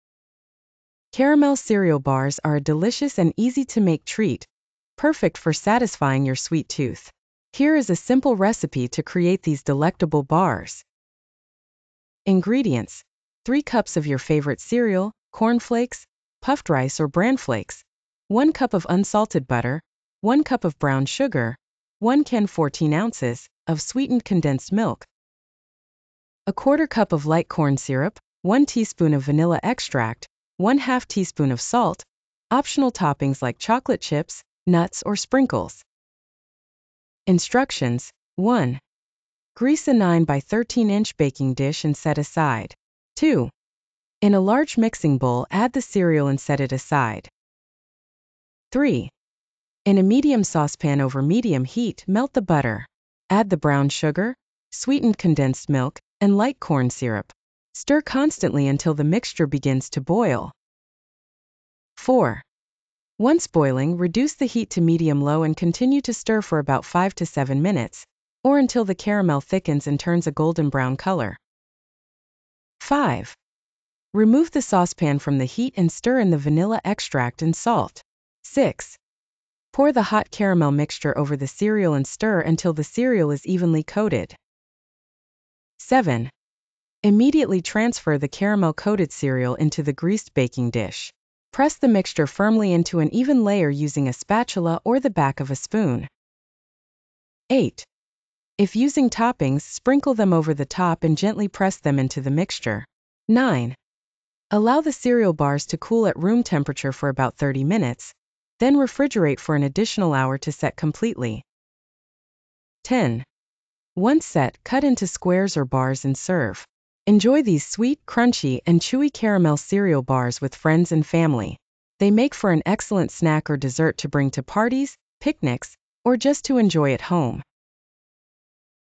Easy Caramel Cereal Bars Recipe
This recipe for caramel cereal bars combines your favorite cereal with a rich caramel mixture made from butter, brown sugar, sweetened condensed milk, and corn syrup. The bars are easy to make and customizable with optional toppings like chocolate chips or nuts. Perfect for parties or a sweet snack at home, these bars are crunchy, chewy, and sure to satisfy your sweet tooth.

Ingredients: 3 cups of your favorite cereal (cornflakes, puffed rice, or bran flakes),
1 cup of unsalted butter,
1 cup of brown sugar,
1 can (14 ounces) of sweetened condensed milk,
1/4 cup of light corn syrup,
1 teaspoon of vanilla extract,
1/2 teaspoon of salt, optional toppings like chocolate chips, nuts, or sprinkles.

Instructions:
1. Grease a 9x13 inch baking dish and set aside.

2. In a large mixing bowl, add the cereal and set it aside.

3. In a medium saucepan over medium heat, melt the butter. Add the brown sugar, sweetened condensed milk, and light corn syrup. Stir constantly until the mixture begins to boil.

4. Once boiling, reduce the heat to medium-low and continue to stir for about 5-7 minutes, or until the caramel thickens and turns a golden-brown color.

5. Remove the saucepan from the heat and stir in the vanilla extract and salt.

6. Pour the hot caramel mixture over the cereal and stir until the cereal is evenly coated.

7. Immediately transfer the caramel-coated cereal into the greased baking dish. Press the mixture firmly into an even layer using a spatula or the back of a spoon.

8. If using toppings, sprinkle them over the top and gently press them into the mixture.

9. Allow the cereal bars to cool at room temperature for about 30 minutes, then refrigerate for an additional hour to set completely.

10. Once set, cut into squares or bars and serve. Enjoy these sweet, crunchy, and chewy caramel cereal bars with friends and family.
They make for an excellent snack or dessert to bring to parties, picnics, or just to enjoy at home.